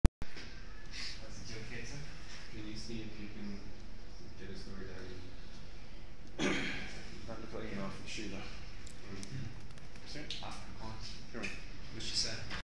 HTC Windows Phone 8X Full HD Video Recording Sample
Full HD video recorded with the HTC Windows Phone 8X at the South African launch